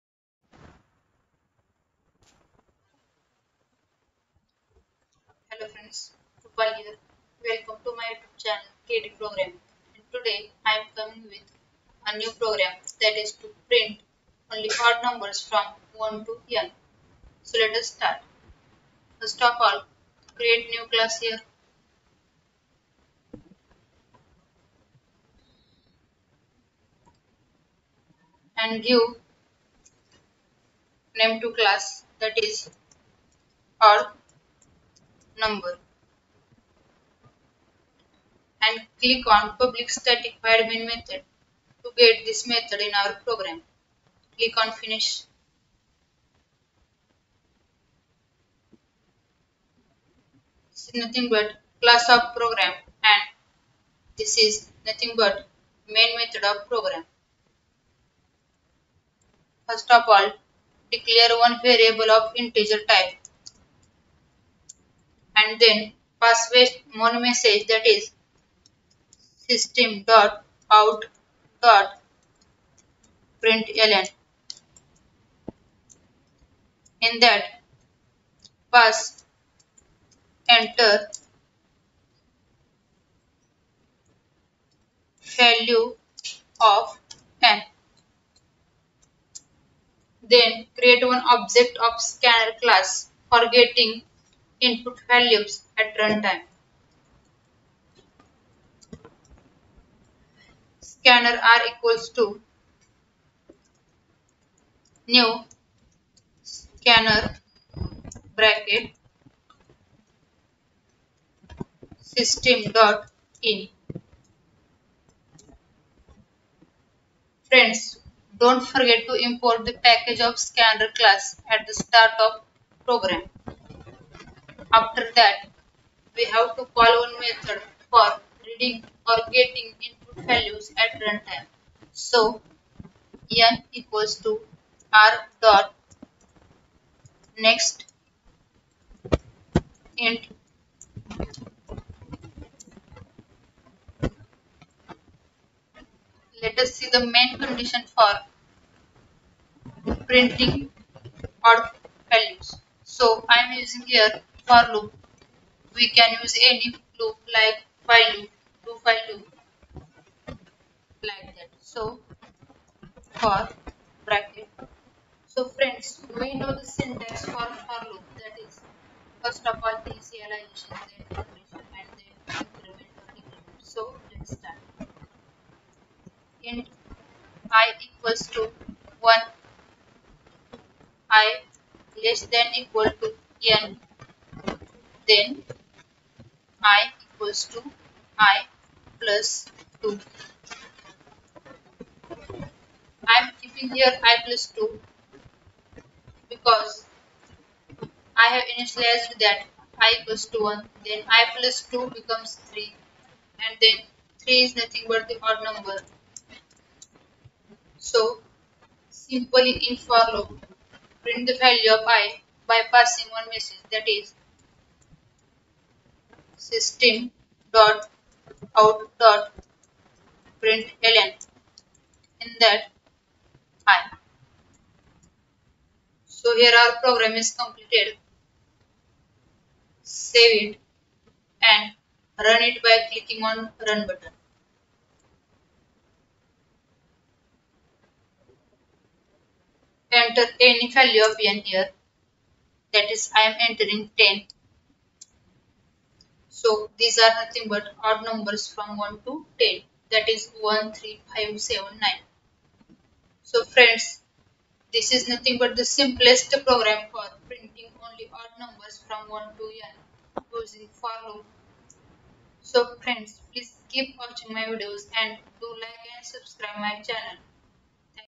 Java program to print odd numbers from 1 to N using for loop
java program to print only odd numbers from 1 to N by using for loop and also come to know the syntax of for loop and how to keep condition in for loop